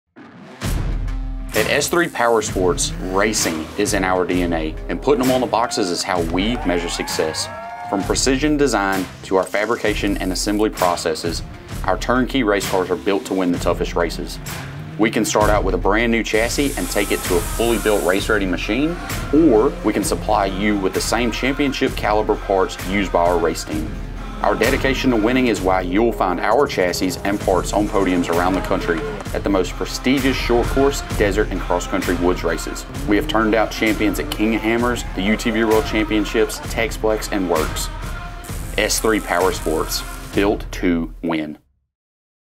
Built to Win: Turnkey Race Cars | S3 Power Sports
Built to Win! From concept to race ready, we build Turn Key Race Cars to spec for all the major race series, customized to meet your needs & show your style!